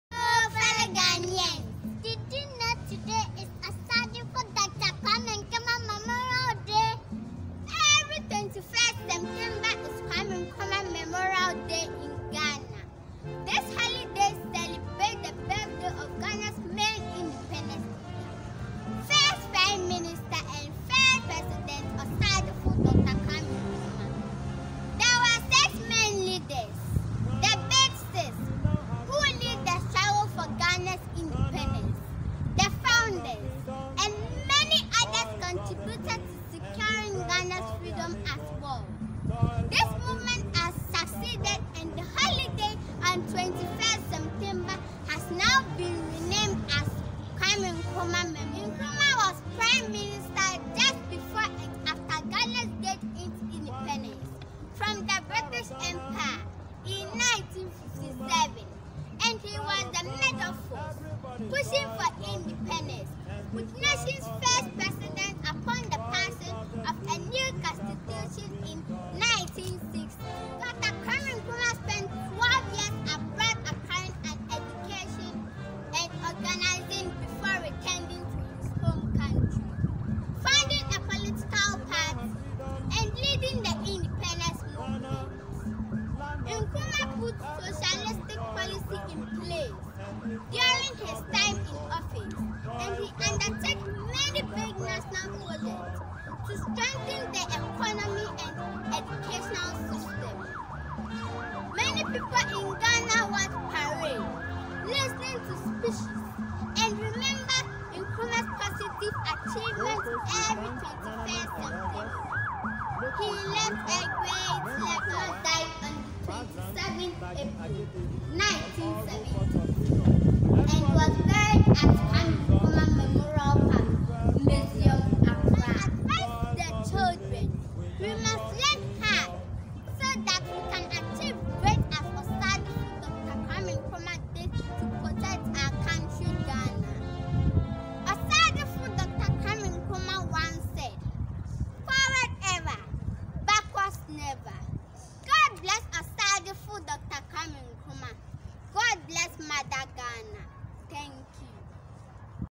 Happy Kwame Nkrumah memorial day 🇬🇭🇬🇭🇬🇭
Today is holiday in Ghana🇬🇭🇬🇭 every 21st September is Osagyefo Dr.Kwame Nkrumah birthday and memorial Day God bless 🙏 Kwame Nkrumah God bless 🙏 Mother Ghana🇬🇭🇬🇭🇬🇭